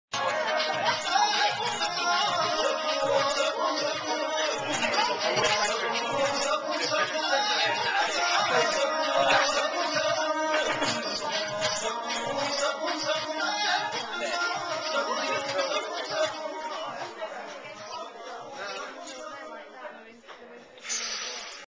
Billiardo
Billiardo in Rotterdam

Italian Game - Played by people from surinam - Took me a bit to work out the rules